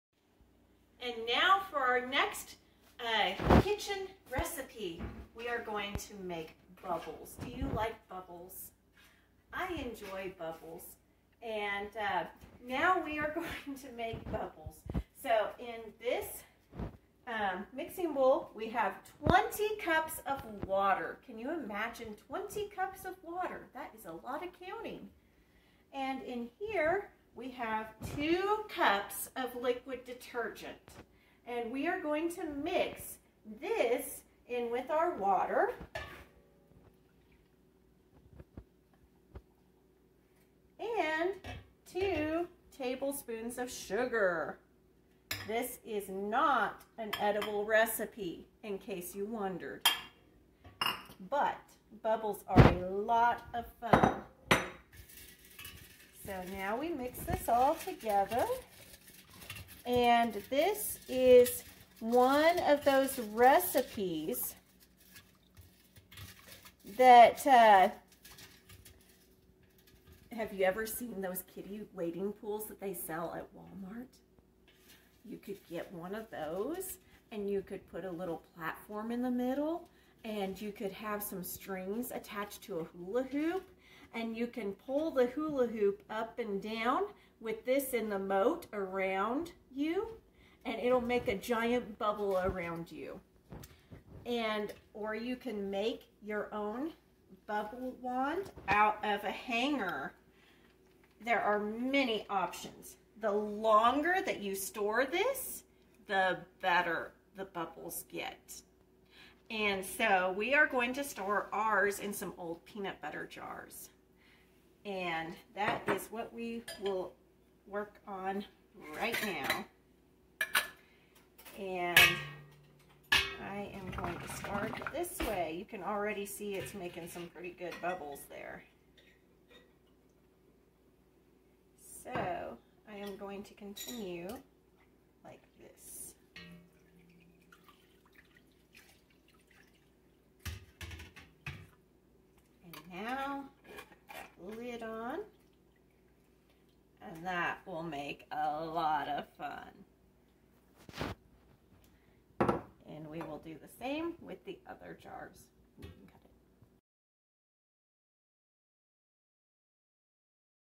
Kids in the Kitchen Bubbles
While this is not an edible recipe, it is still very fun recipe that could save a lot of money Have fun and stay safe!